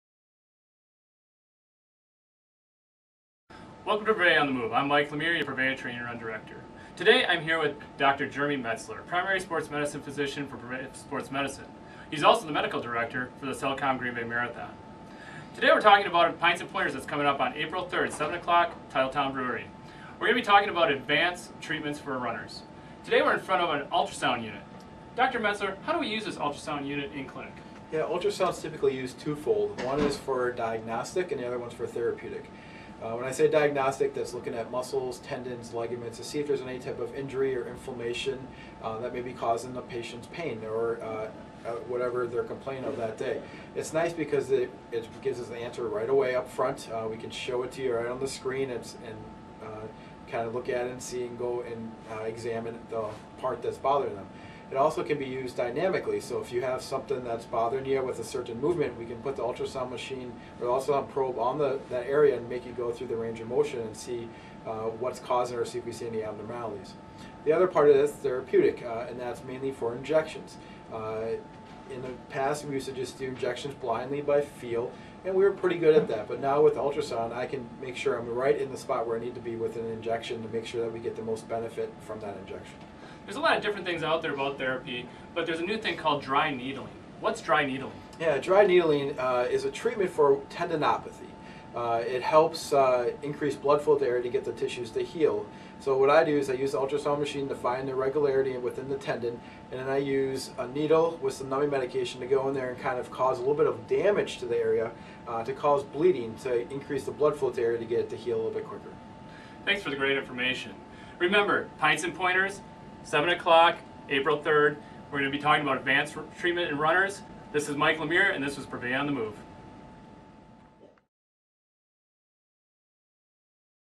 Prevea on the Move - Advanced Treatment for Runners
Prevea Pints & Pointers: Advanced Treatment for Runners

Have you ever given thought to just how far sports medicine treatments have come--even throughout the last decade? The latest sports medicine treatments and preventive measures available to reduce the chance of injury during training are extraordinary. Some of these treatment options include: Musculoskeletal Ultrasound imaging, PRP treatments and Kinesio taping. This week's Prevea On the Move offers a sneak peak at the latest and greatest in advanced treatments from Cellcom Green Bay Marathon Medical Director and Prevea Primary Care Sports Medicine physician Dr. Jeremy Metzler.

Mark your calendars on Wednesday, April 3 to get answers about these advanced treatment options.
7 to 8 p.m.
Titletown Brewing Co., Second Floor
200 Dousman St., Green Bay

Prevea Pints & Pointers is a free series with expert speakers offering tips and answering questions about running and preparing for the marathon.